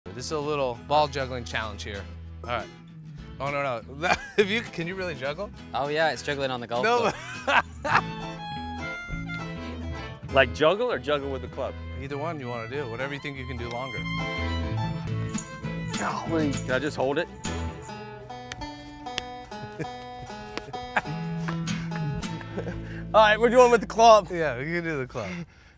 Behind-the-Scenes Shenanigans | Presented by Travelers Championship
All the behind-the-scenes shenanigans you didn't know you needed, presented by the Travelers Championship.